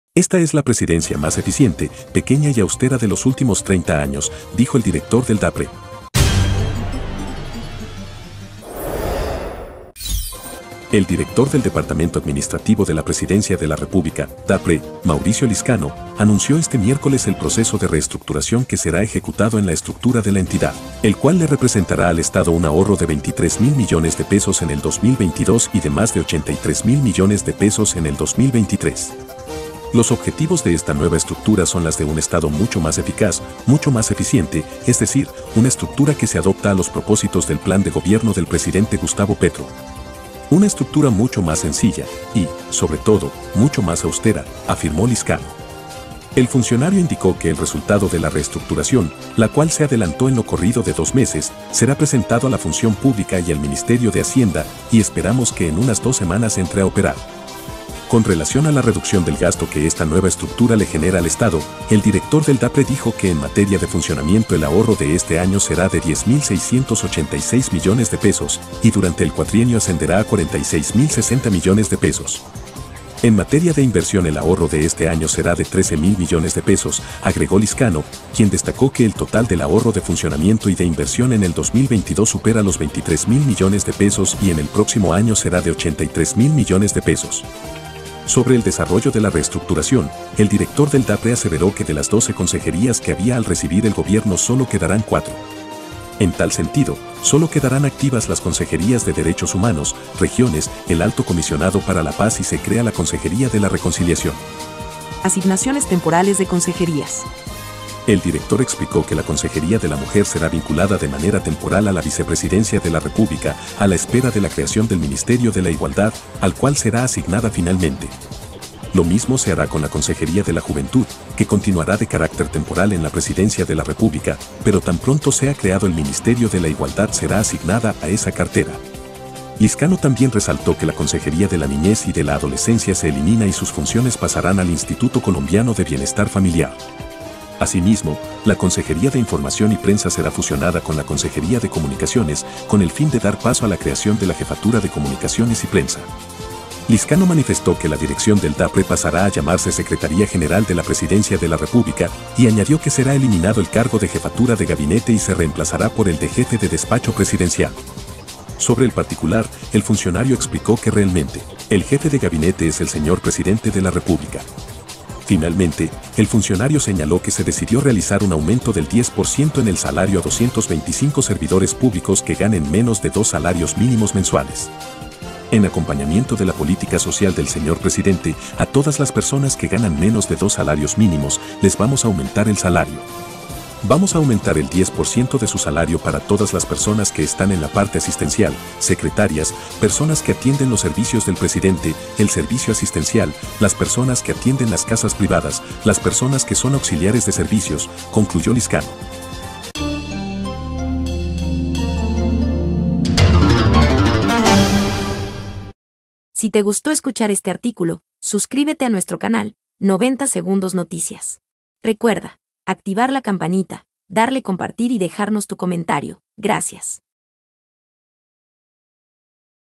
⭕El Estado se ahorraría $10.600 millones con la reducción de las consejerías, según Dapre 👇👇
El director del Departamento Administrativo de la Presidencia de la República (Dapre) de Colombia, Mauricio Lizcano, anunció este miércoles que la Presidencia reducirá de doce a cinco sus consejerías, y algunas de ellas serán temporales a la espera de la creación del Ministerio de igualdad.👇👇
90 Segundos Noticias, Somos independientes, con neutralidad e imparciales, objetivos y honestos en su concepto estricto. Sin prejuicios, justos y equilibrados. ...